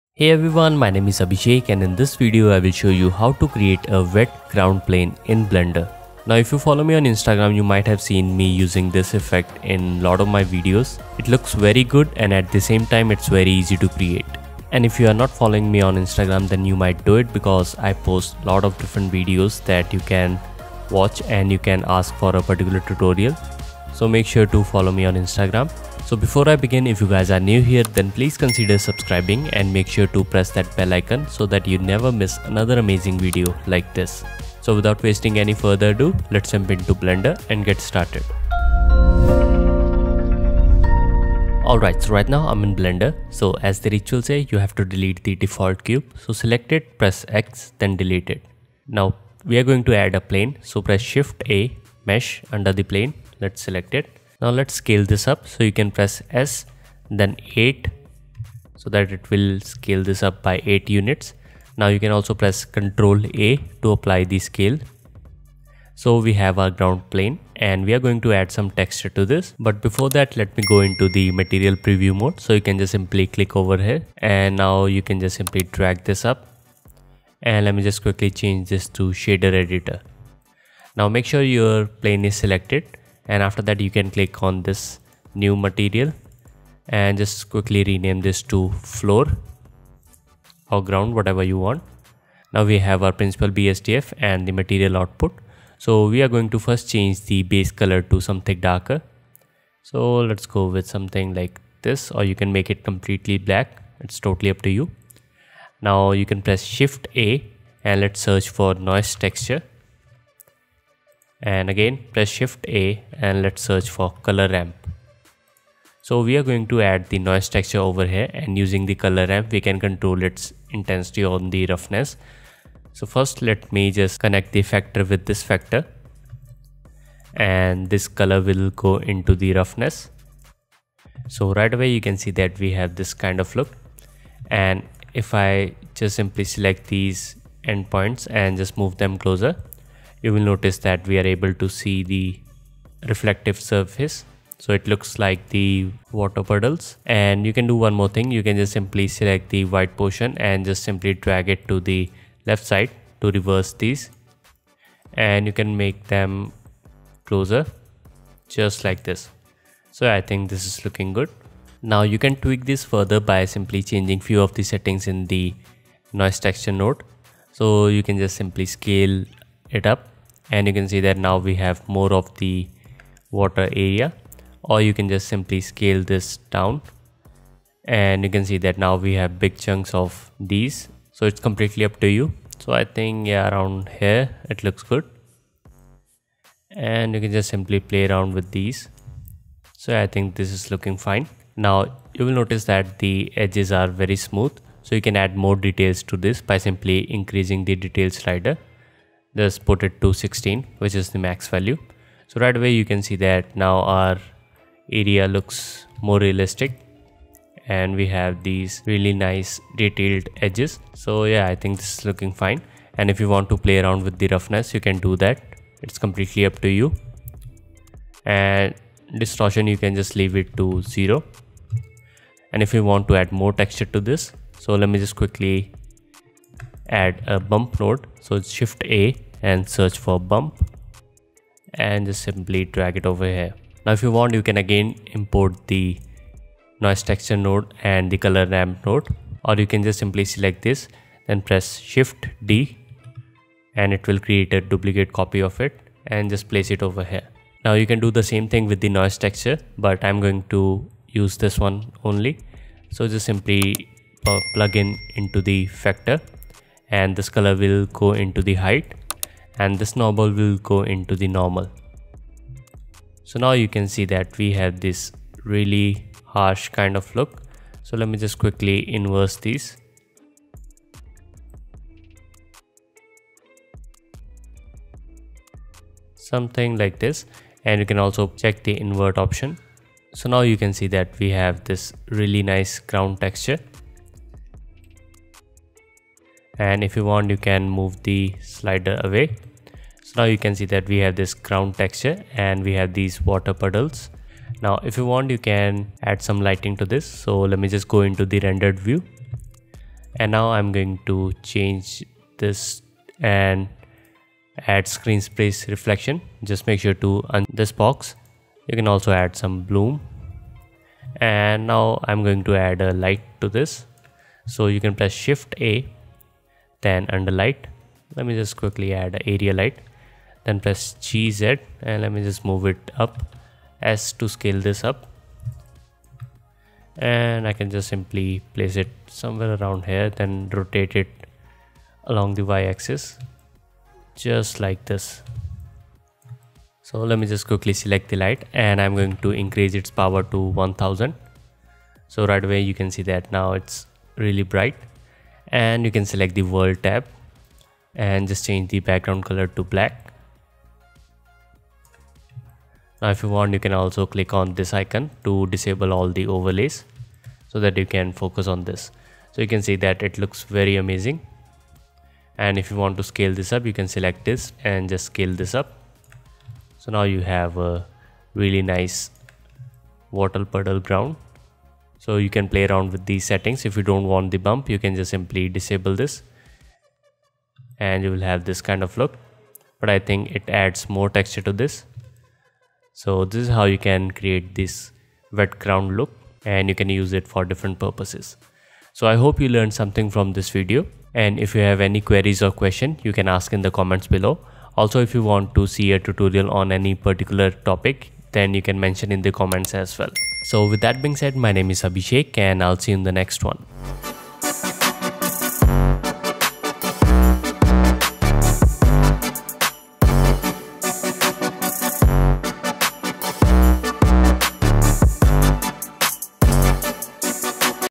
Realistic Wet Ground Texture in Blender - Eevee & Cycles | Blender 2.8 Tutorial | Beginners
Create Realistic Wet Ground Texture in Blender for free. Using this easy method you can have a realistic wet floor in just a few seconds.

Motion Nations